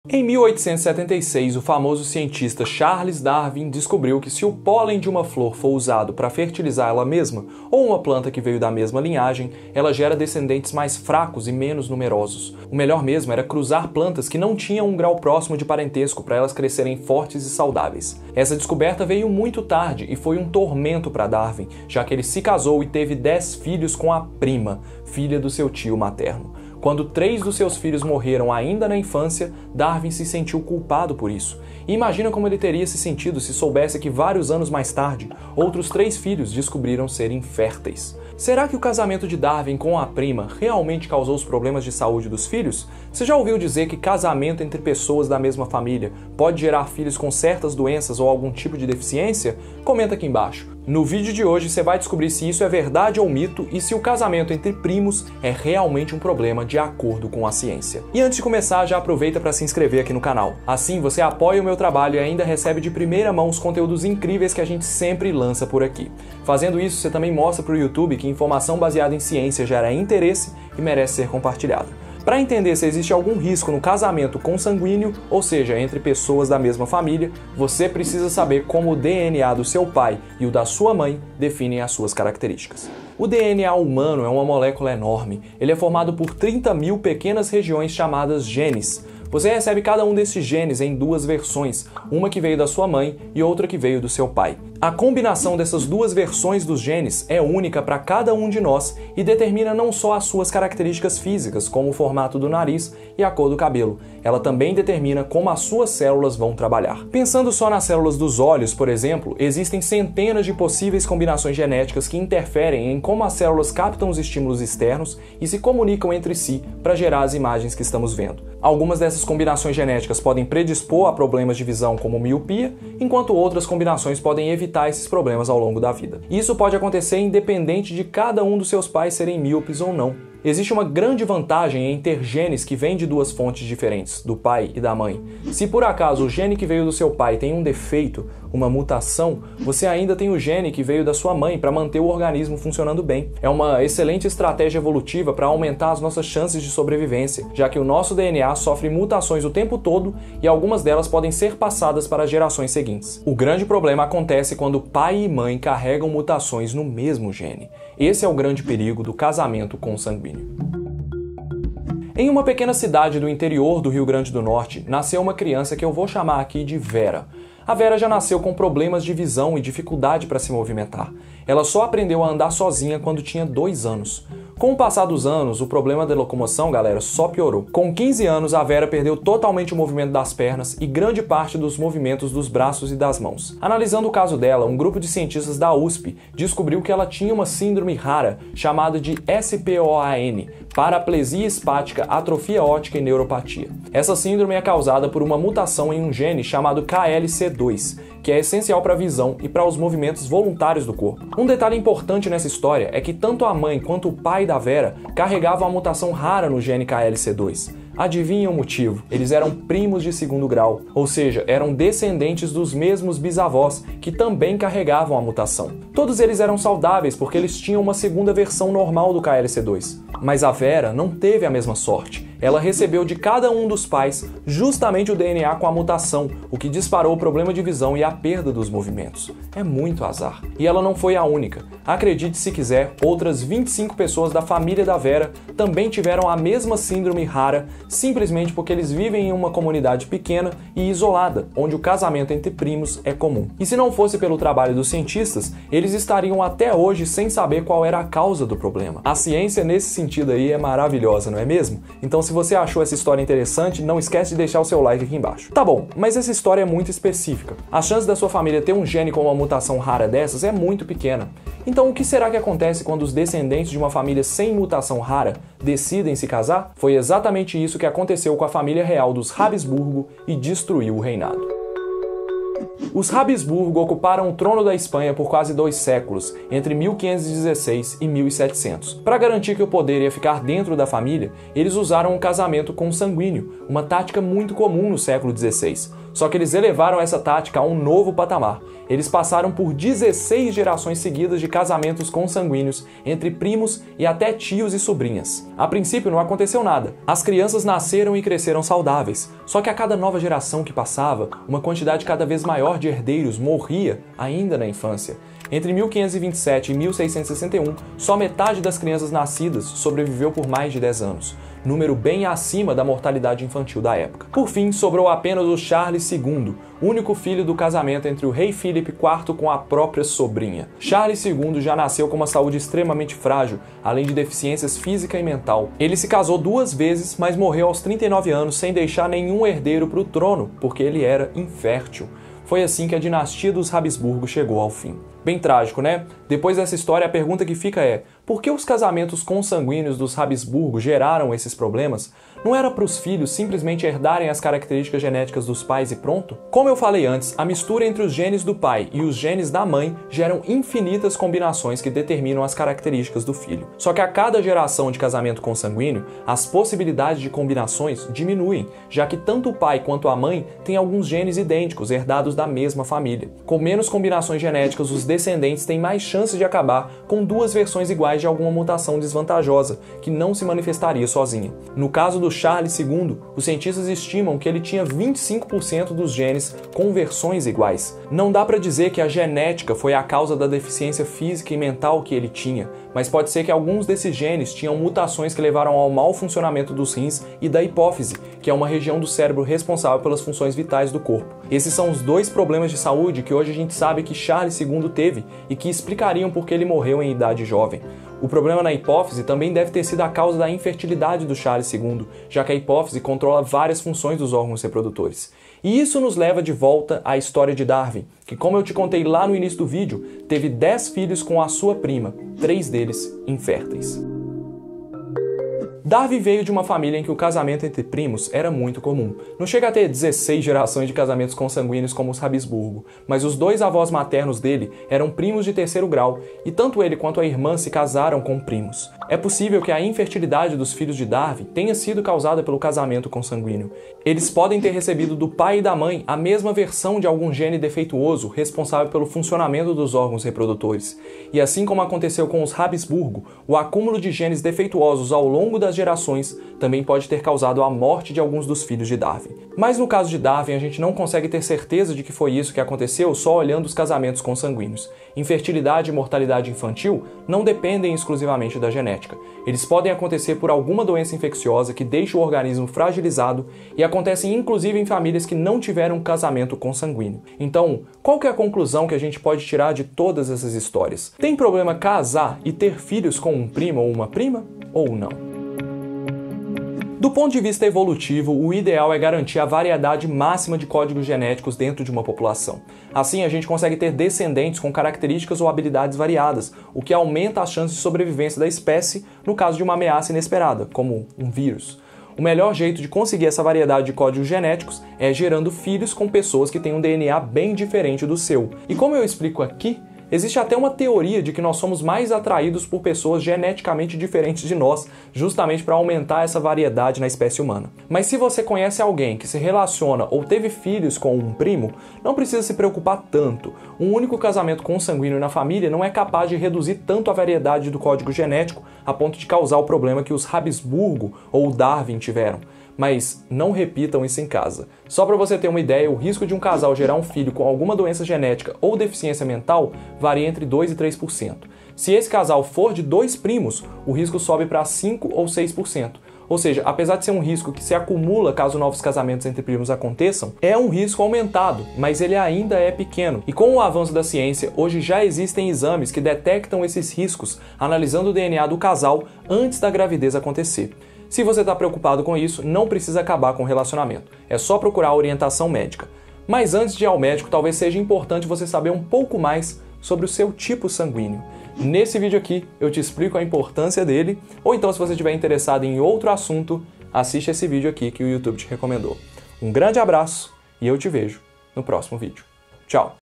O TRÁGICO DESFECHO DE SE CASAR COM SEU PRIMO
O casamento consanguíneo acontece quando duas pessoas da mesma família têm um relacionamento, inclusive quando ocorre um casamento entre primos. A seleção natural, proposta por Darwin, promete um trágico desfecho para uma família que insiste nessa prática por muitas gerações. Se essas pessoas tiverem filhos, as consequências podem ser terríveis. Descubra no vídeo de hoje o real perigo de ter filhos com um primo, do ponto de vista da genética. E o próprio Darwin pode ter deixado um legado terrível ao ter tido mais de 10 filhos com sua prima.

Direção e Apresentação: Lucas Zanandrez
Roteiro: Bruna T. Maria
Edição: Vinícius Marangon